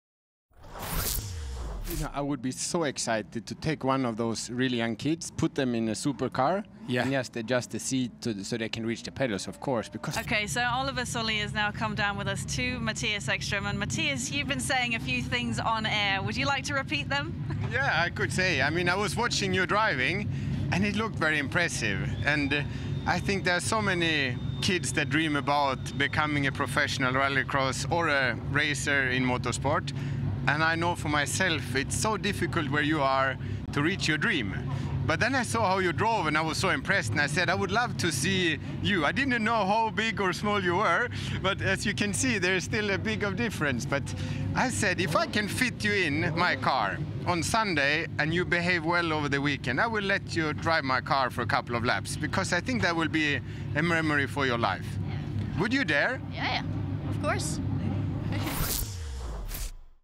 MATTIAS EKSTRÖM INVITES KID TO DRIVE SUPERCAR
Mattias Ekström was so impressed with Olliver Sollies Crosscar Jr driving that Oliver got a invite to drive Mattias Supercar. This we want to see!